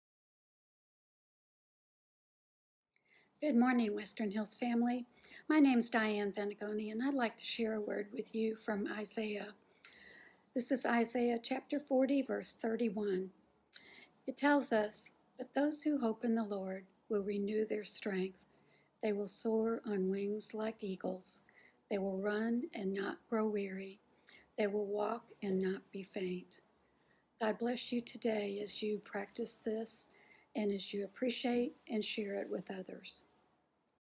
9 Encouraging Word, 08/13/2020
Diane Venegoni shares an ENCOURAGING WORD for you.